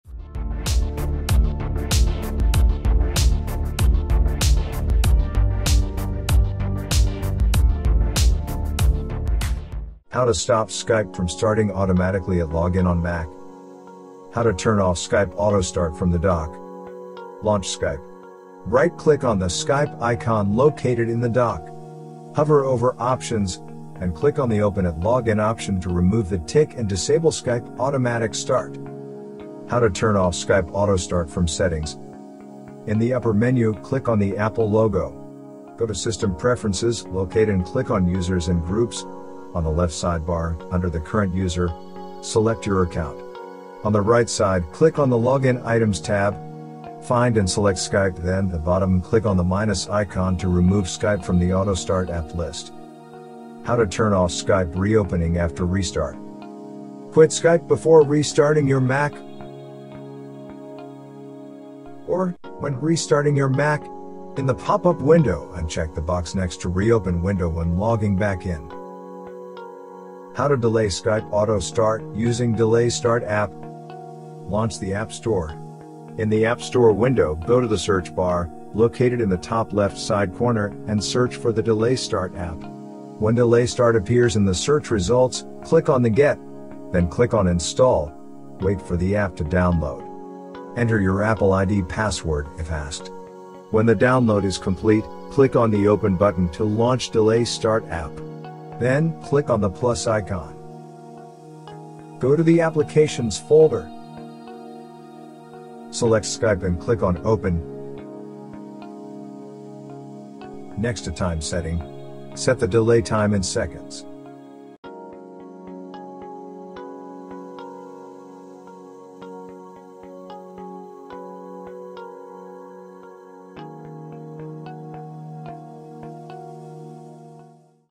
How to Stop Skype from Starting Automatically at Login on Mac?
Video showing how to Stop Skype from Starting Automatically at Login on Mac.